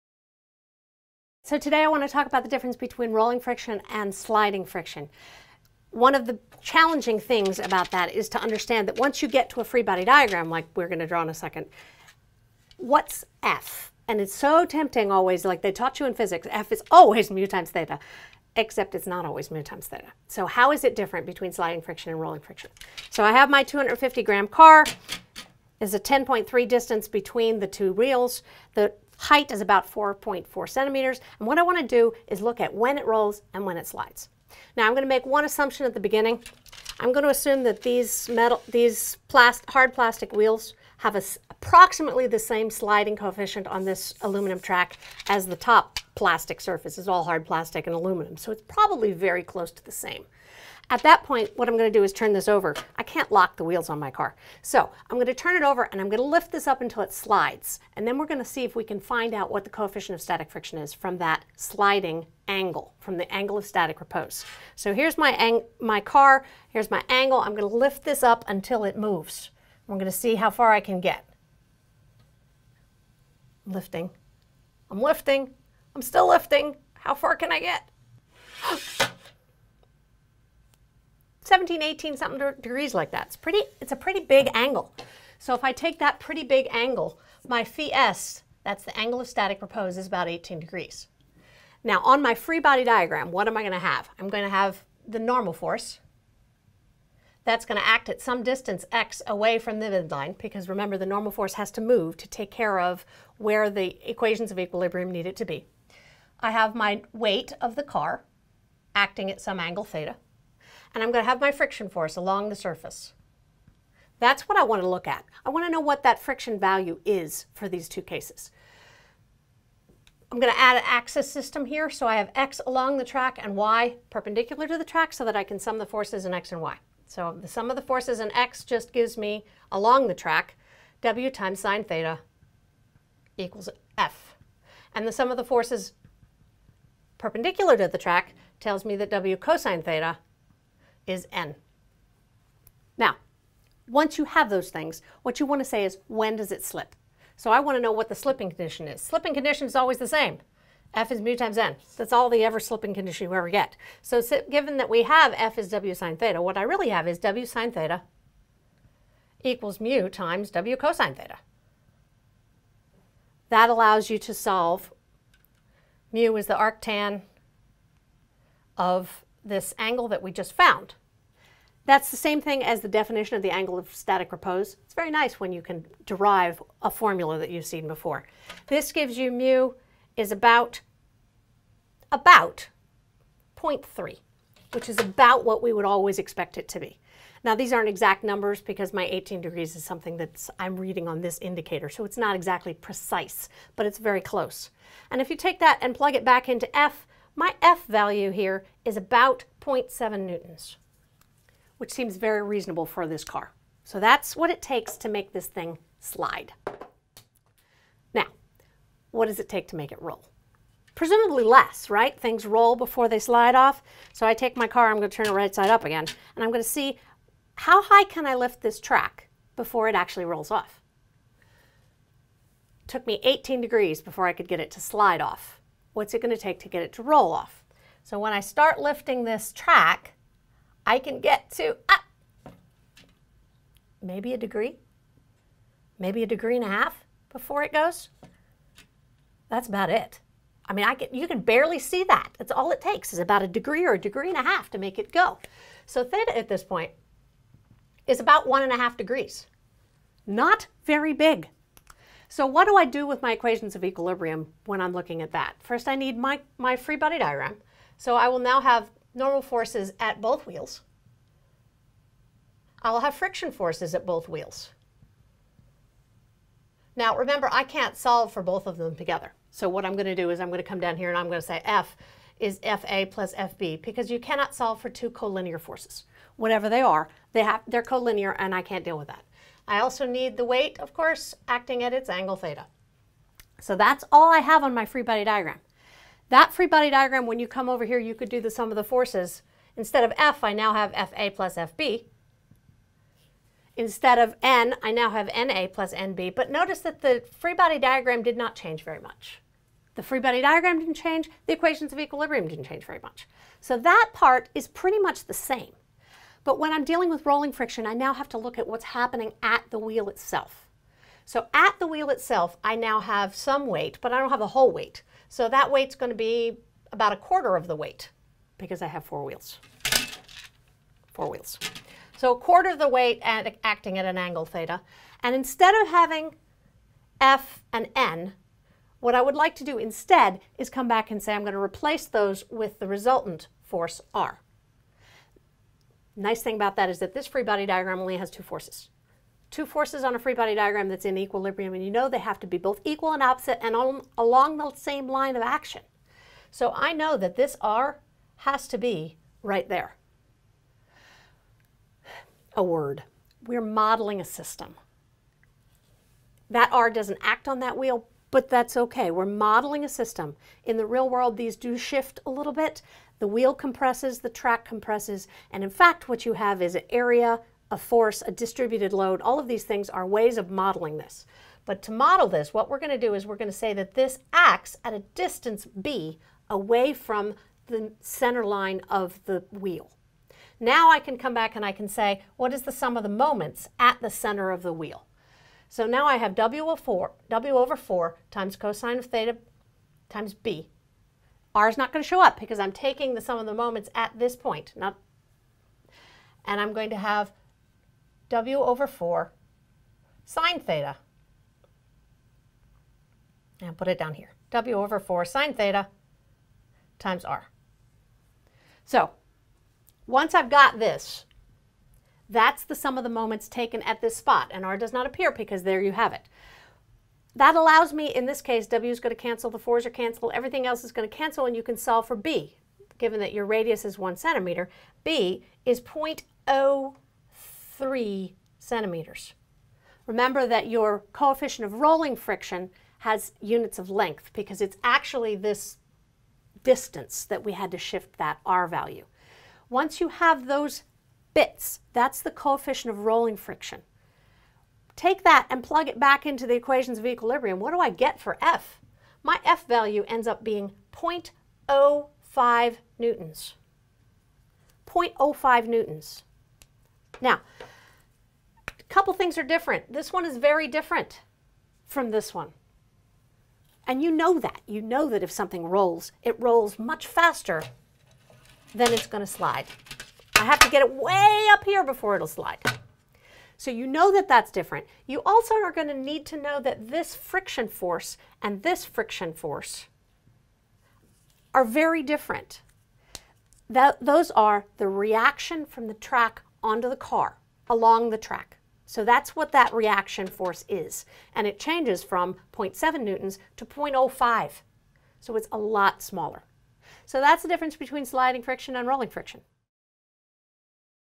Rolling vs Sliding Friction
demonstration showing rolling friction calculations vs sliding friction calculations
with special thanks to Stembrite.org!